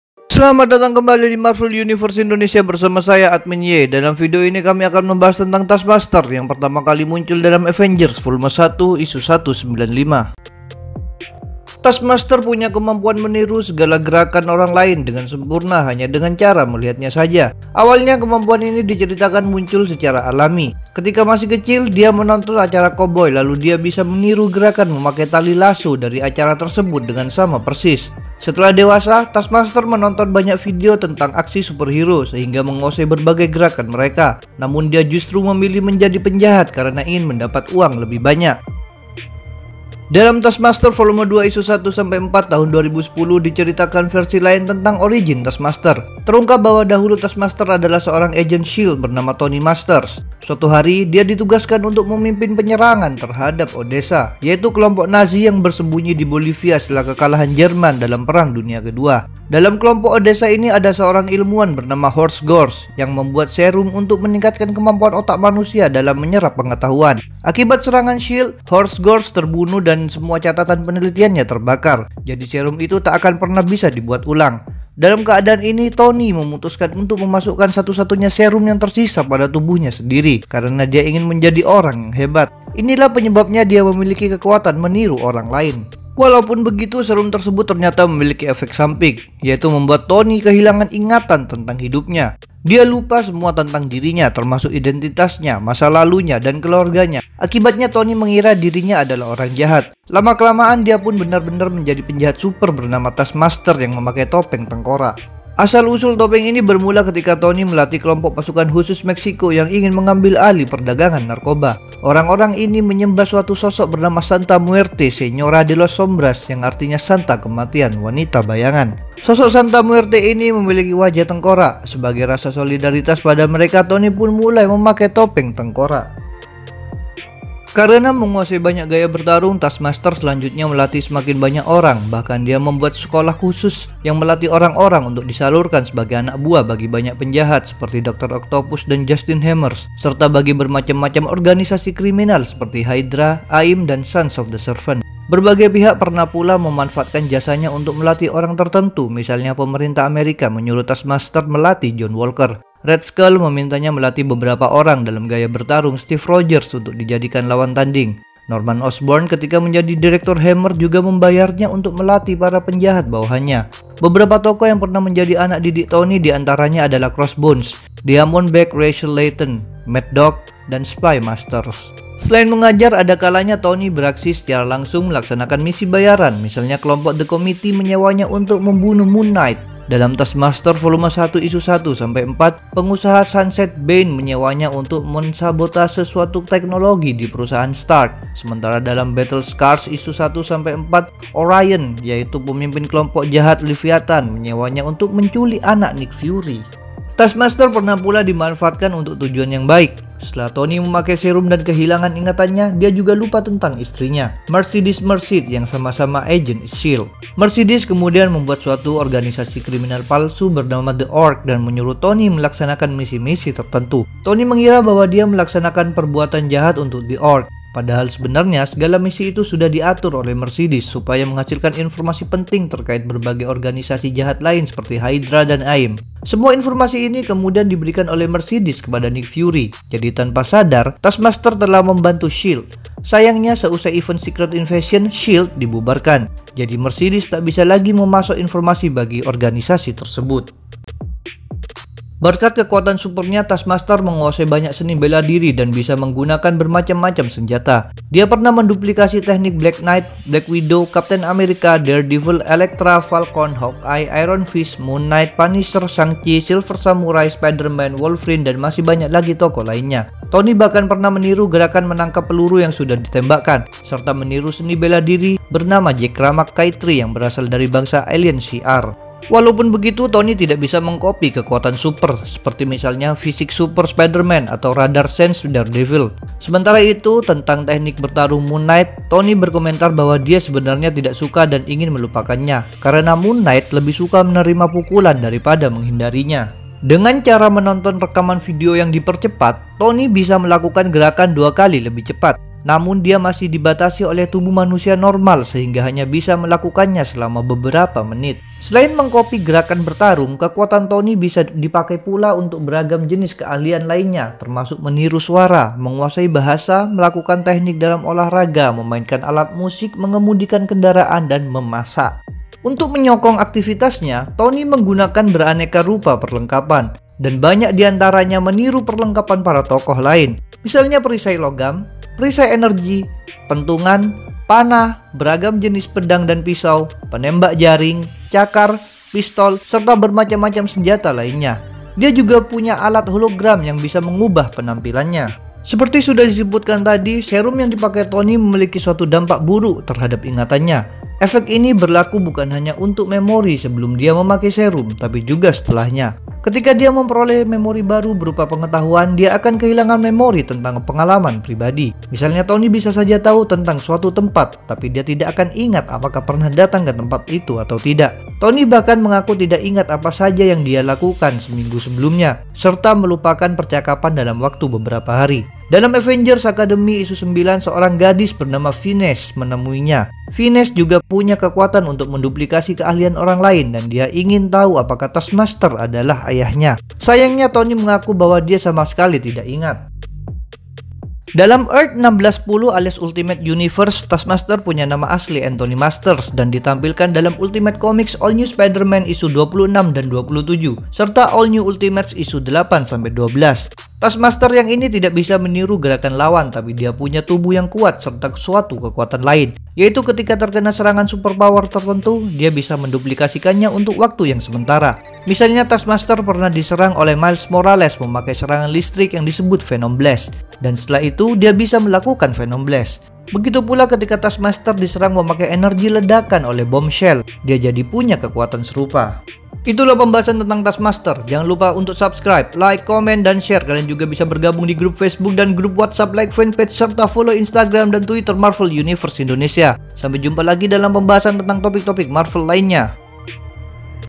Sang Peniru Keahlian | Asal-Usul Taskmaster (Tony Masters)
Dalam video yang keenam puluh lima ini, Marvel Universe Indonesia membahas tentang Taskmaster. Video ini dibuat oleh:

Scriptwriter & Editor:
- Admin A1

Grup Whatsapp MUI Regional

1. Batam

2. Jabodetabek

3. Situbondo - Jember

4. Bandung

5. All Regions